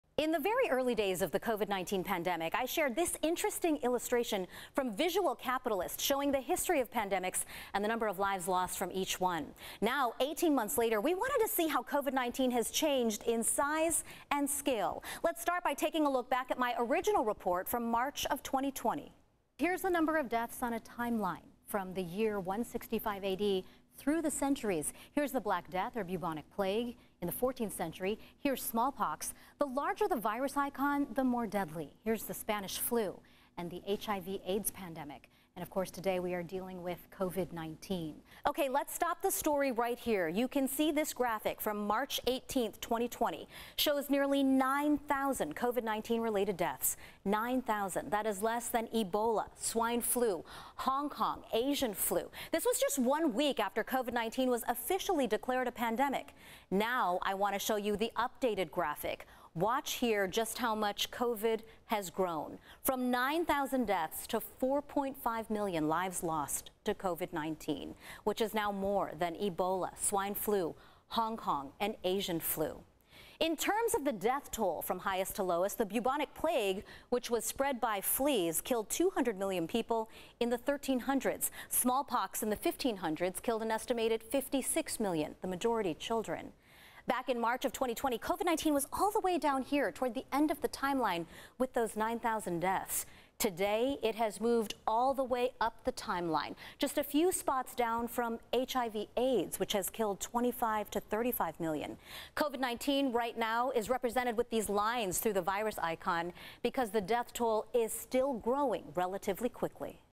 How does COVID-19 compare to some of history's most deadly pandemics as of 2021?
In the very early days of the COVID-19 pandemic, News 8's Marcella Lee shared this interesting illustration from Visual Capitalist showing the history of pandemics and the number of lives lost.

Now, 18 months later, we wanted to see how COVID-19 has changed in size and scale.